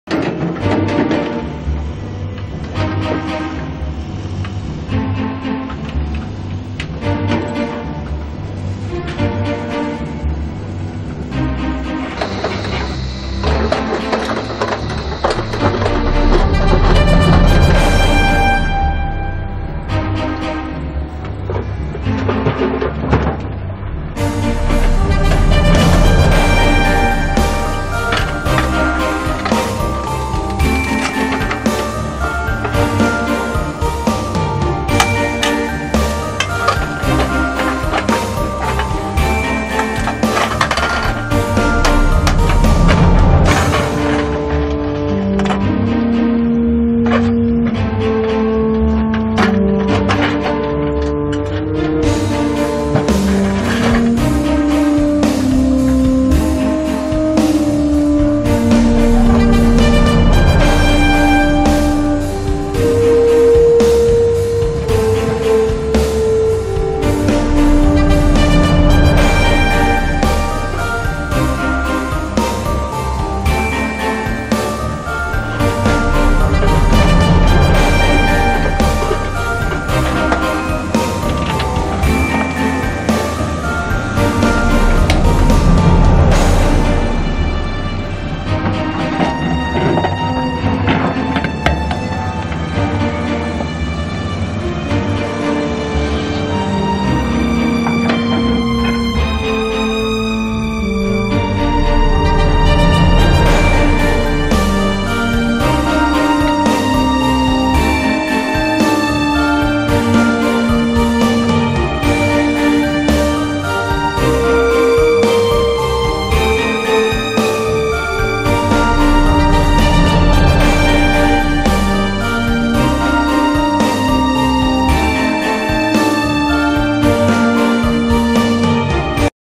Tuscany: Preparation of soil for a new vineyard in Gaville